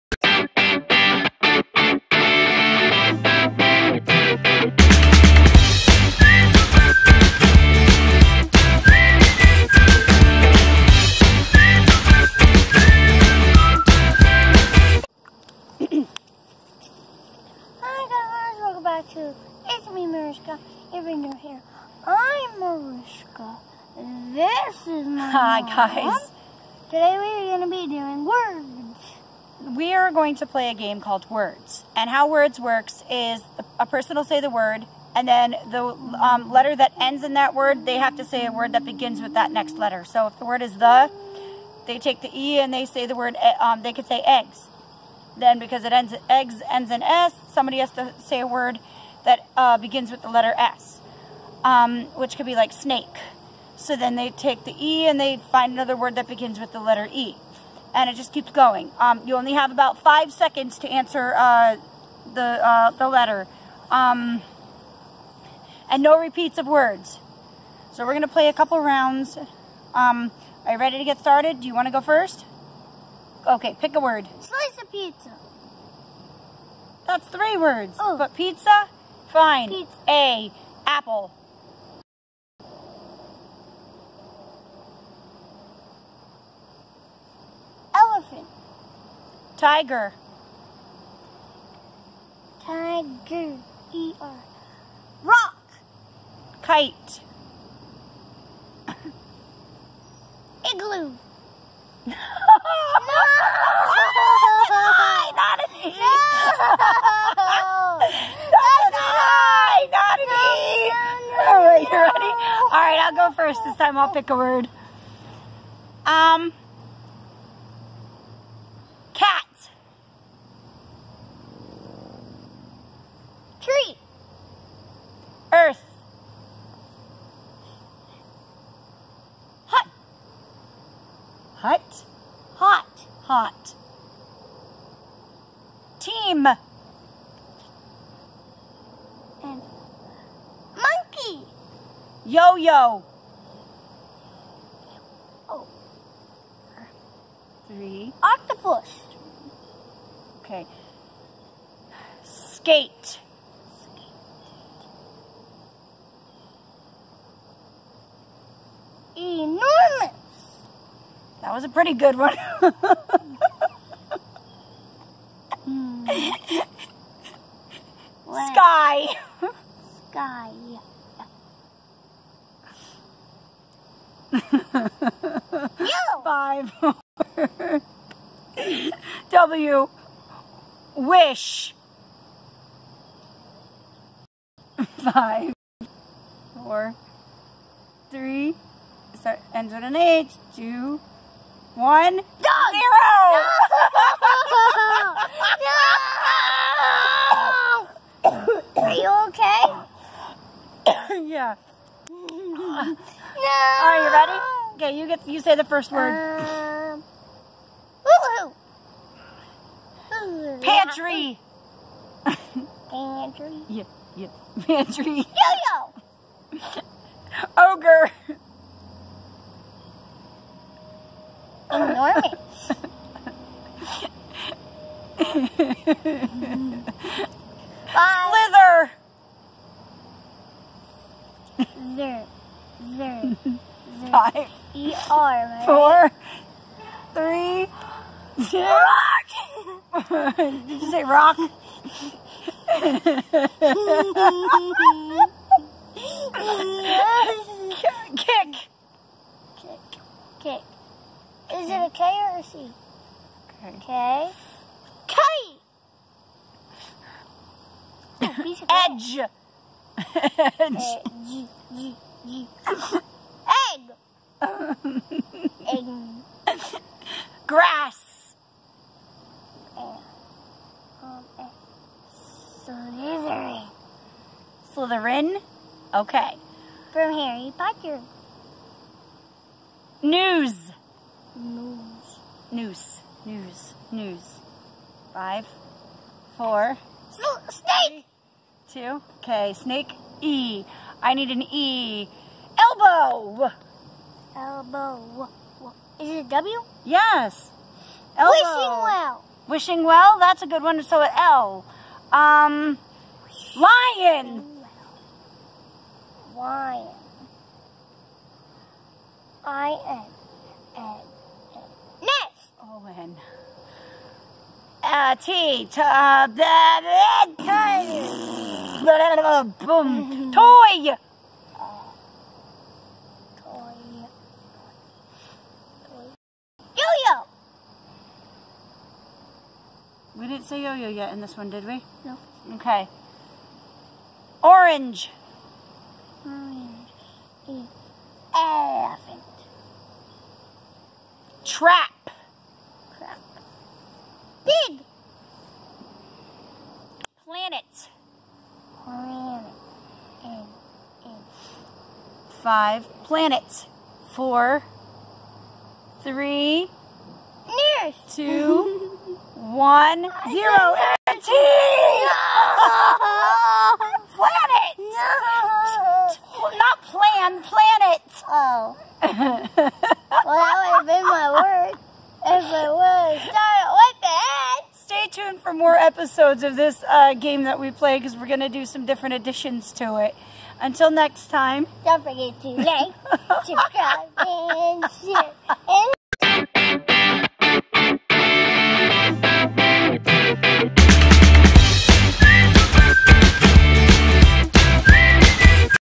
Words (A Hilarious Game to Say Anything)/ItsM3Mariska
We play a game called words. This game is so simple all you do is say a word (dog the say a word that starts with the last letter g goat and so on, no repeats and 5 seconds to say the word).

💙Mariska💙